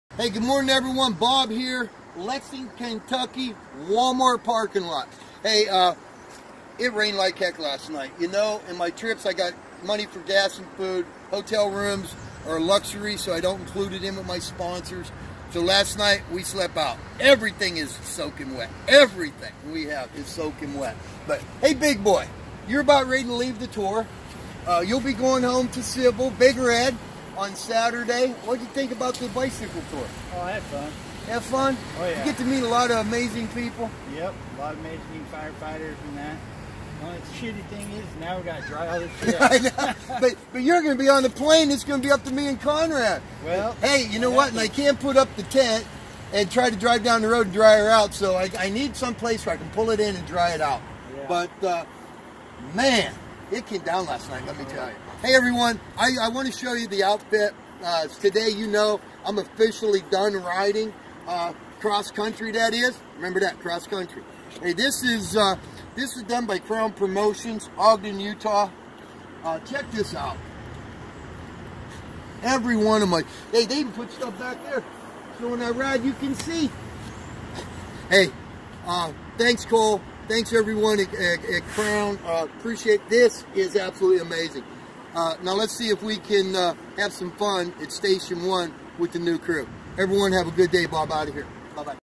Lexington Kentucky Walmart Parking Lot
Lexington Kentucky Walmart Parking Lot Us Saying Good Buy To Big Boy And Other Stuff. TY